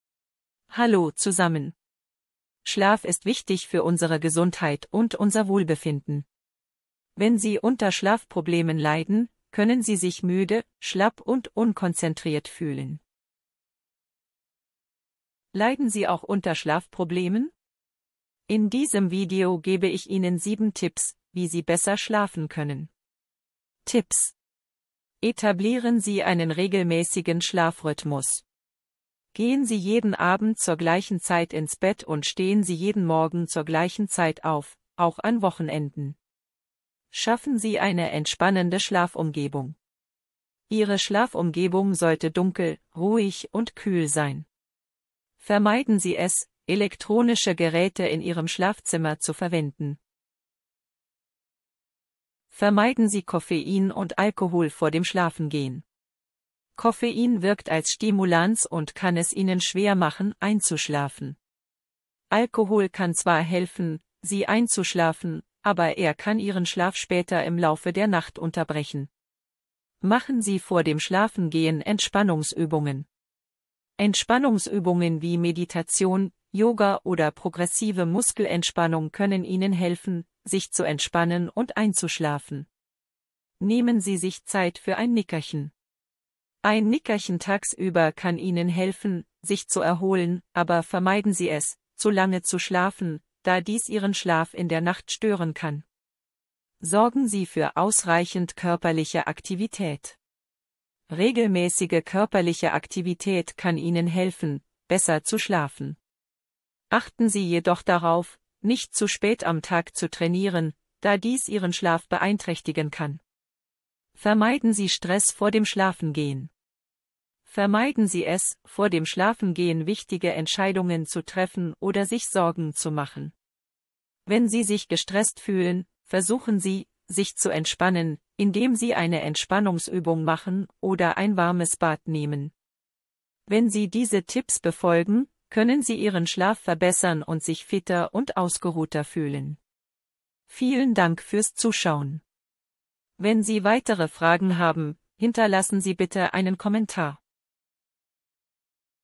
Hast du Schlafprobleme ? 😴😴😴
Schlaf ist wichtig für unsere Gesundheit und unser Wohlbefinden. Er ermöglicht uns, uns zu erholen, zu lernen und unser Gedächtnis zu festigen. Wenn wir unter Schlafproblemen leiden, kann sich dies negativ auf unsere Gesundheit und unser Leben auswirken.
Was sind Schlafprobleme?
Schlafprobleme werden auch als Schlafstörungen bezeichnet. Sie können sich in verschiedenen Formen äußern, zum Beispiel in Form von Einschlafstörungen, Durchschlafstörungen oder unruhigem Schlaf.